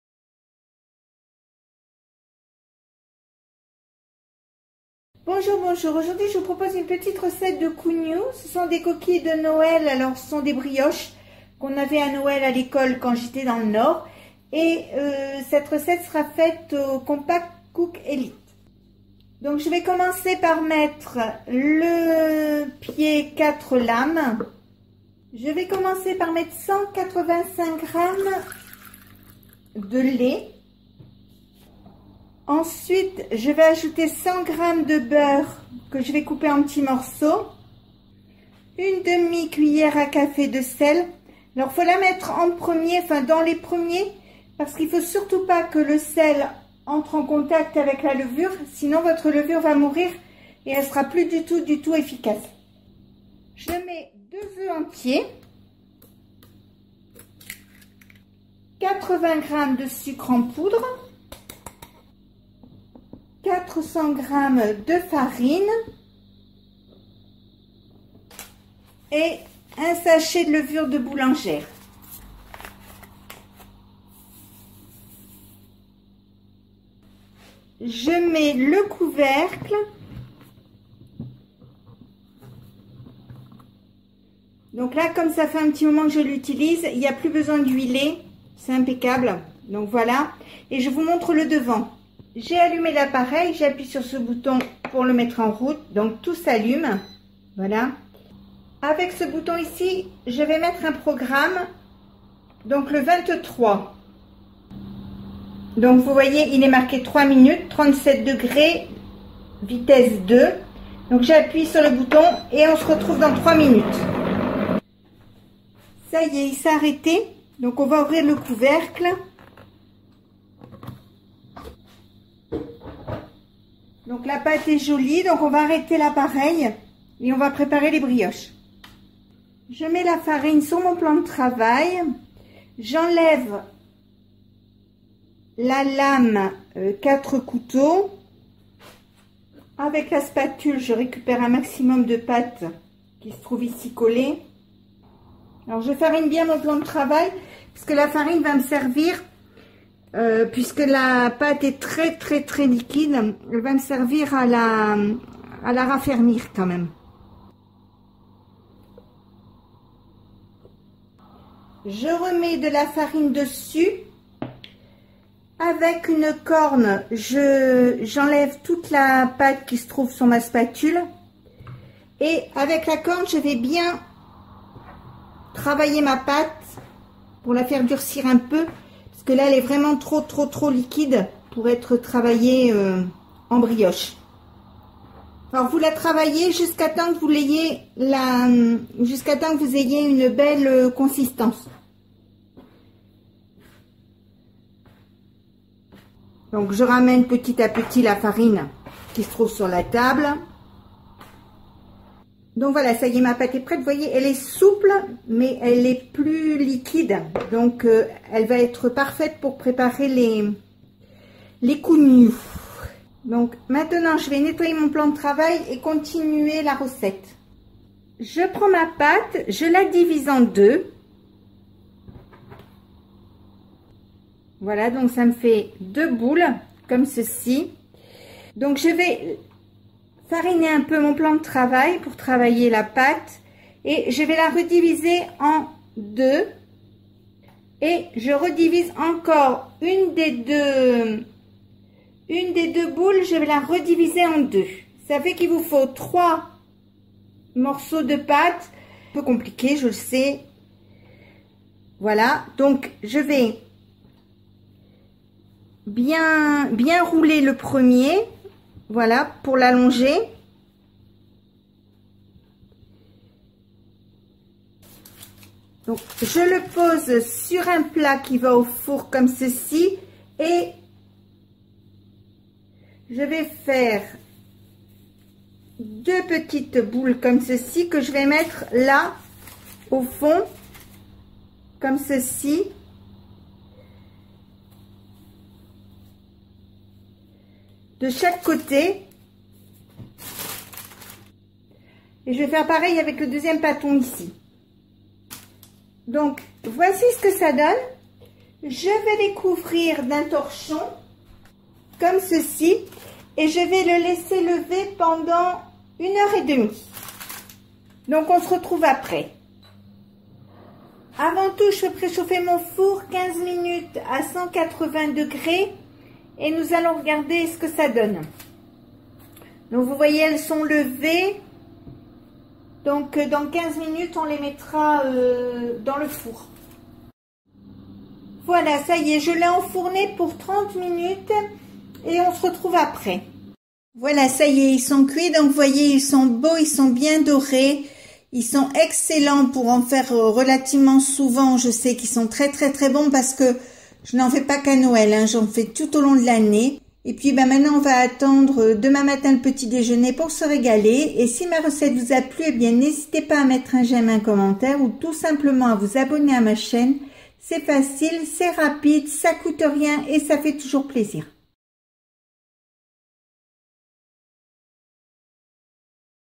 Cougnou au compact cook elite pas à pas
Bonjour, bonjour,
Aujourd'hui je vous propose une petite recette de cougnou au compact cook elite.
Ce sont de délicieuses brioches originaires du nord de la France. Elles sont faciles et rapides à faire.
Ingrédients :
- 185 gr de lait
- 100 gr de beurre
- 1/2 cuillère à café de sel
- 2 oeufs
- 80 gr de sucre en poudre
- 400 gr de farine
- 1 sachet de levure de boulanger ou 20 gr de levure fraîche